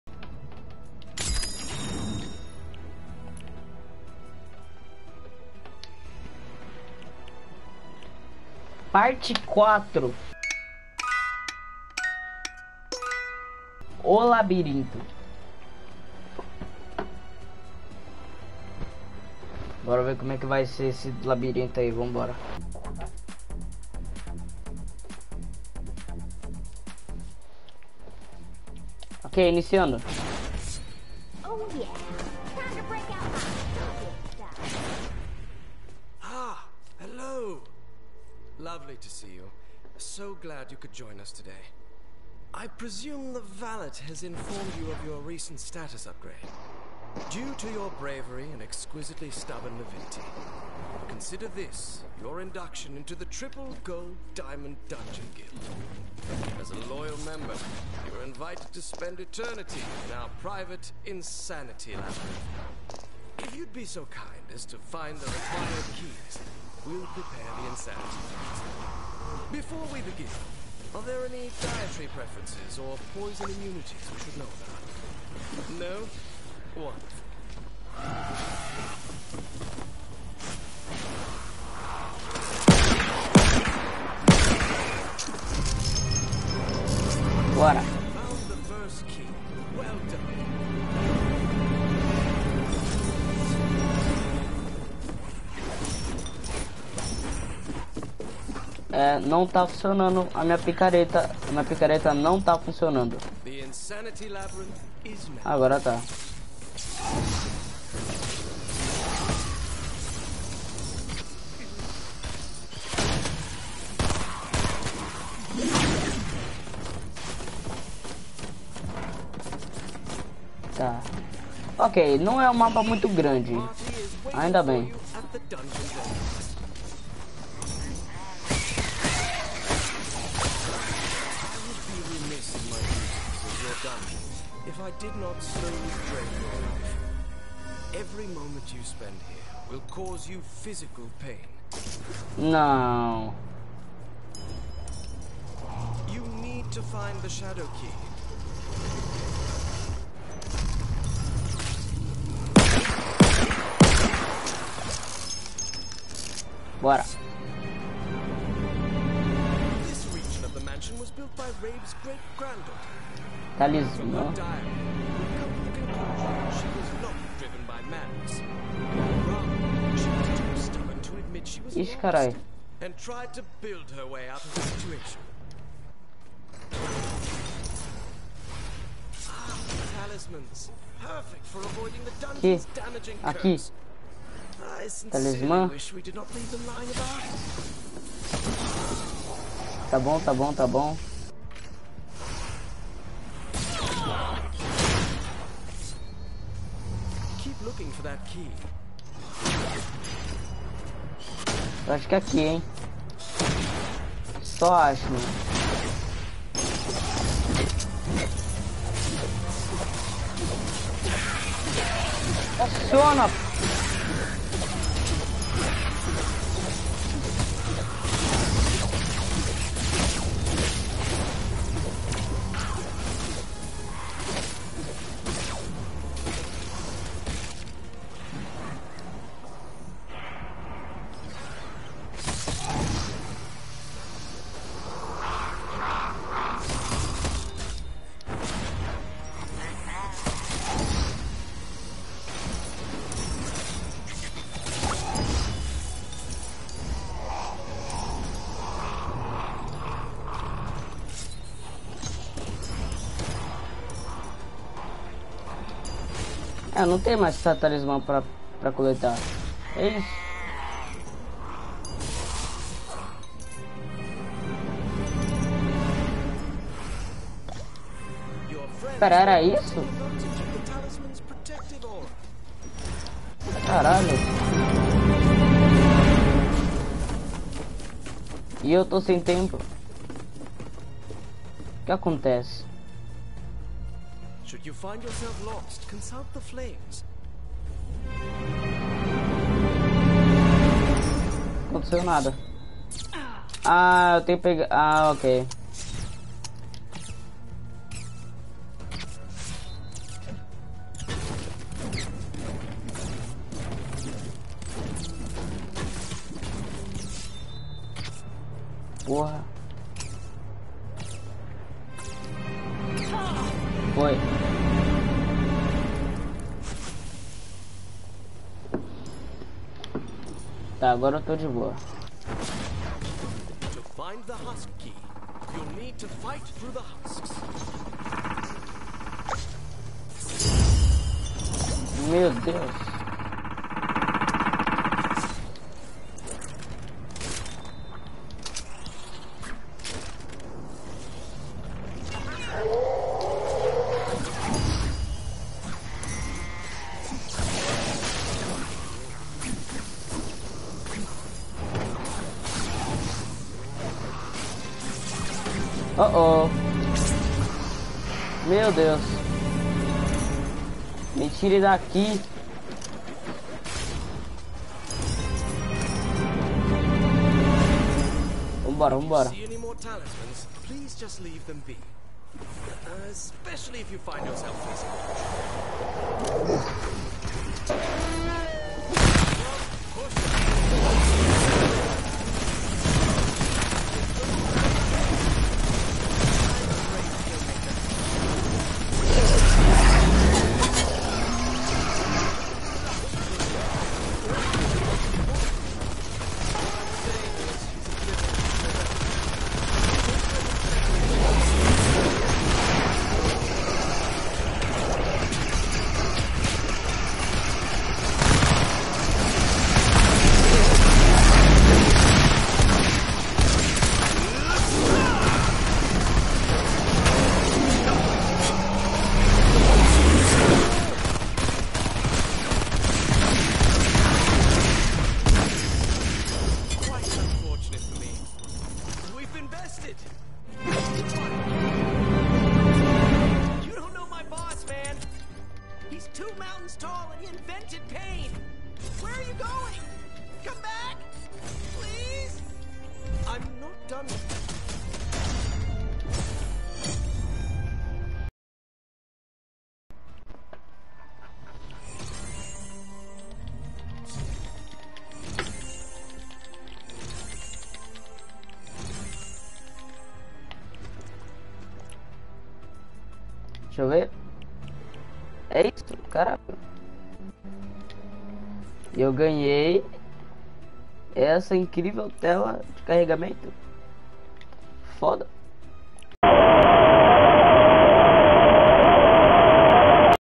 As Masmorras parte 4: O Labirinto
Essa foi a parte 4 das masmorras: o labirinto. Essa é a parte da masmorra mais curta de todos, já que o mapa não é nem grande. Cada parte levou cerca de uns 2 ou 3 minutos, e OLHA QUE É A ÚLTIMA PARTE, mas comentem o que vocês acharam